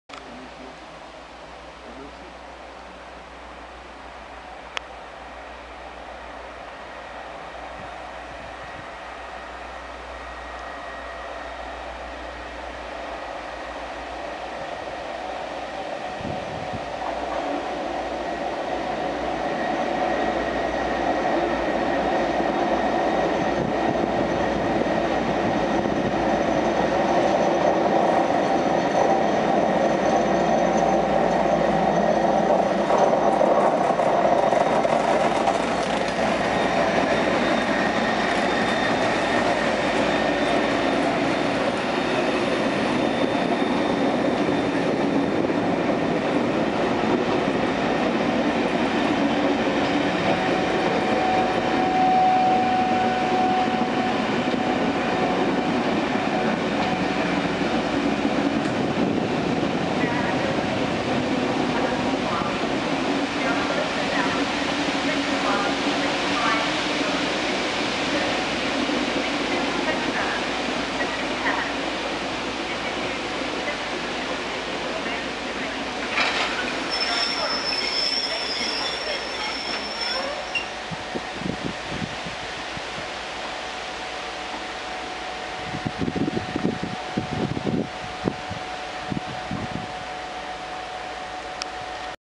57601+47804 'The Northumbrian Fellsman' Sutton Coldfield 25.8.12
A real rarity: a 'proper' train on the Lichfield Trent Valley - Birmingham New Street line!